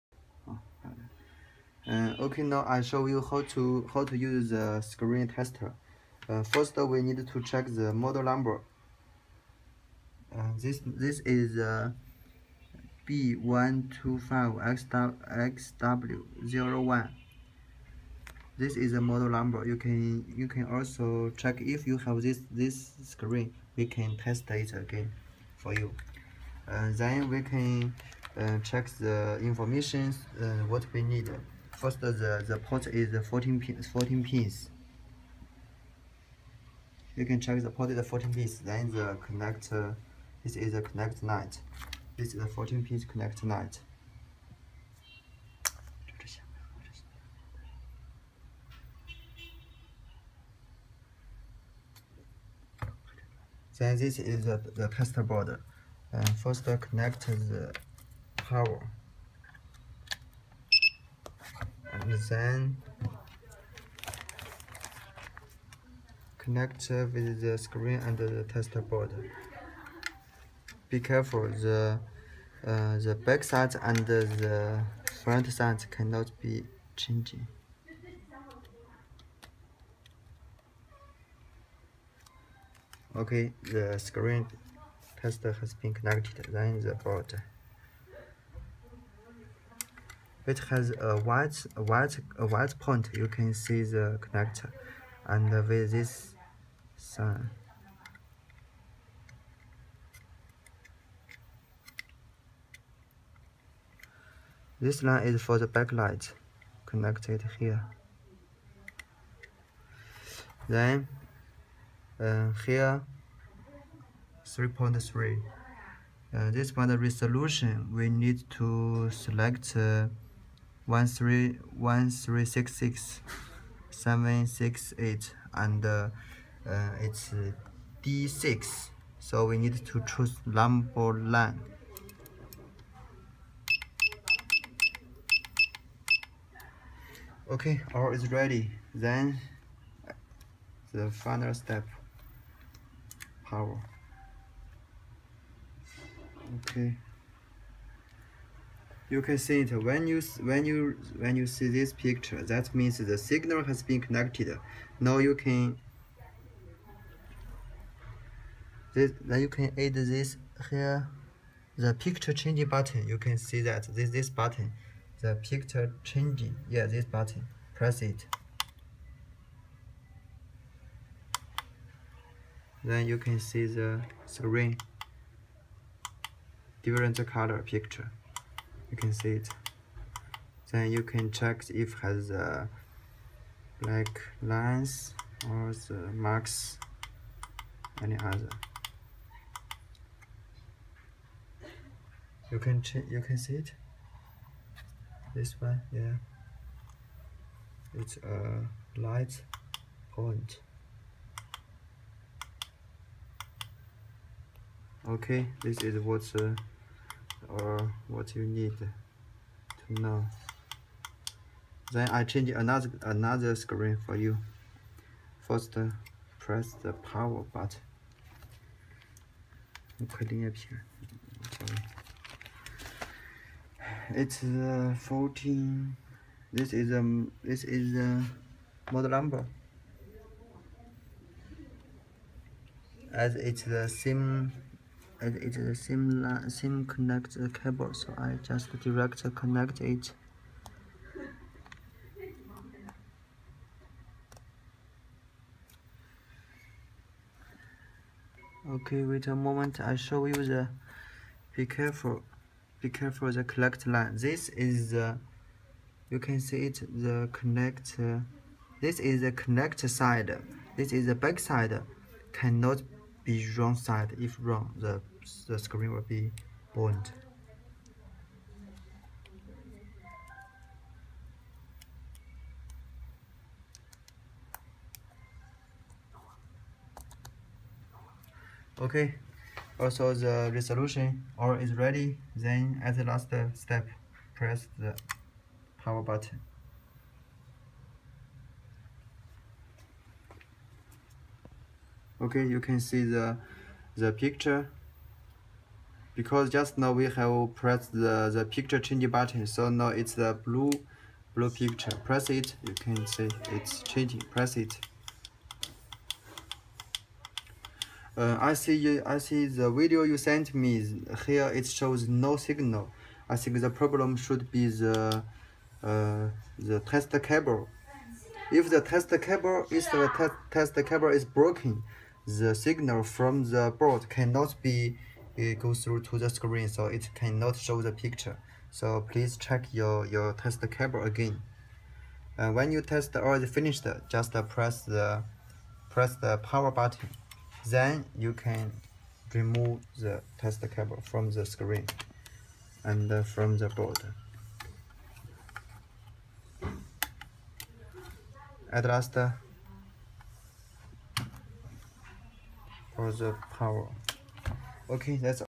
How to use screen tester by lepustech.com
how to use screen tester to test the LCD screen.